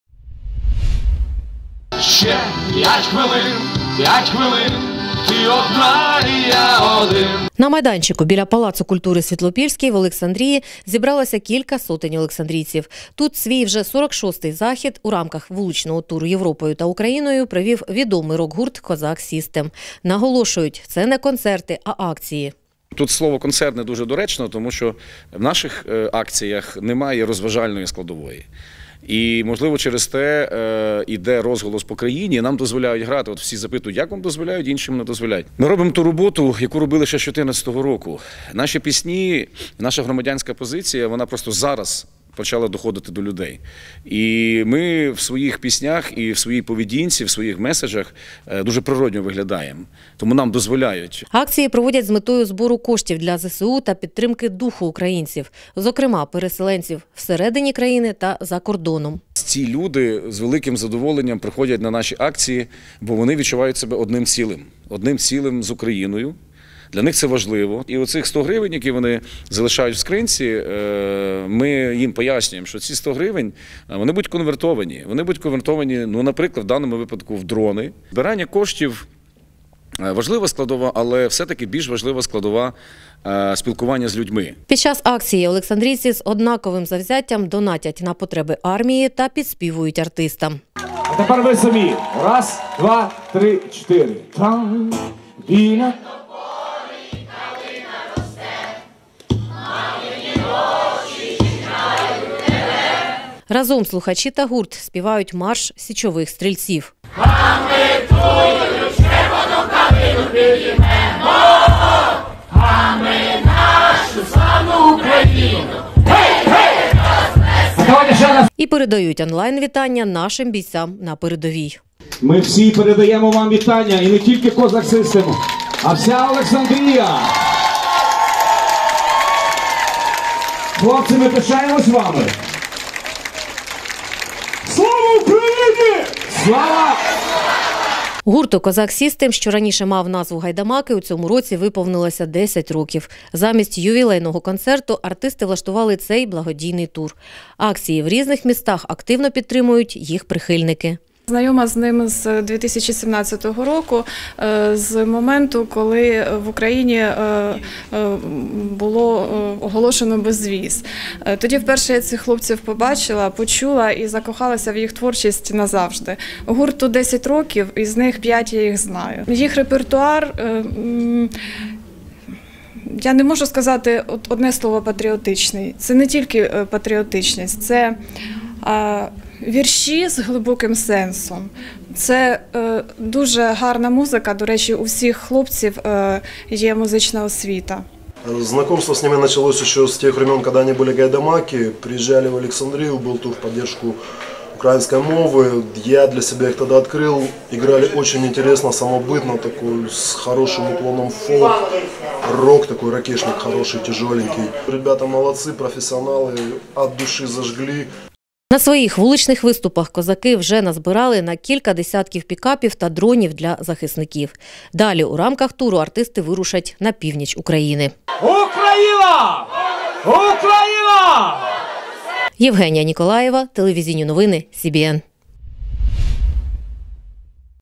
Гурт «KOZAK SYSTEM» завітав до Олександрії | телеканал Вітер
У рамках свого вуличного туру рок-гурт «KOZAK SYSTEM» провів благодійний захід в Олександрії. Просто неба артисти виконали свої композиції для того, щоб зібрати кошти на підтримку ЗСУ. Пісні рокерів чула наша Євгенія Ніколаєва.